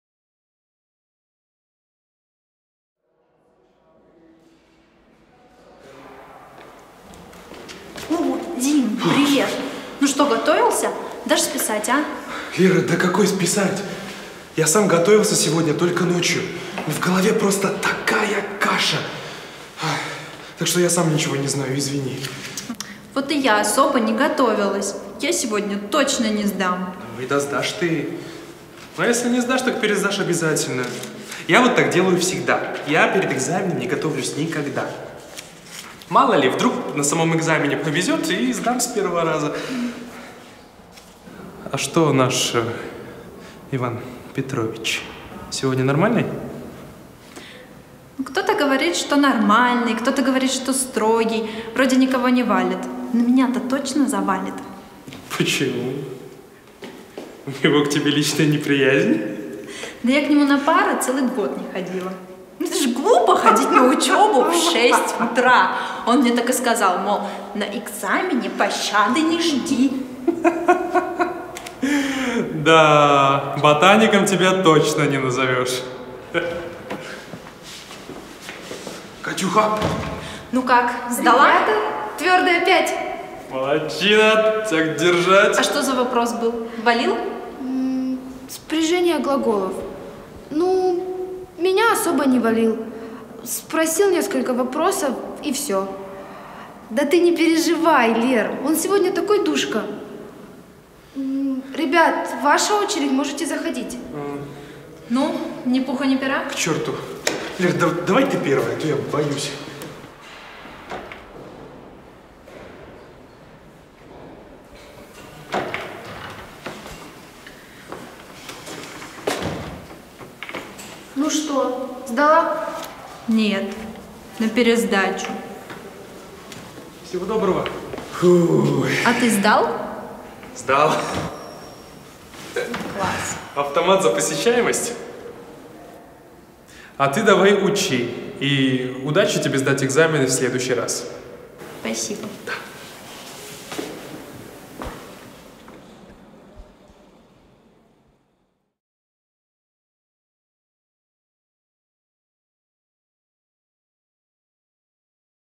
Fred Rj - Zkouška
Jedno z dvaceti dvou výukových videí ruského jazyka vyrobených pro portál Fred který provozuje Nakladatelství Fraus, s.r.o.